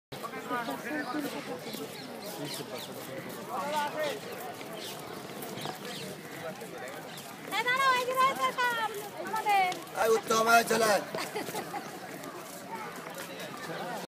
EL TAJ MAHAL AL AMANECER Una experiencia digna de ser vivida... !
Una de las mejores experiencias en la vida, es sin duda, ver el amanecer enel taj mahal, que a pesar de la bruma matutina, nos deja entrever, lo que hoy es considerada, la maxima obra de la arquitectura universal.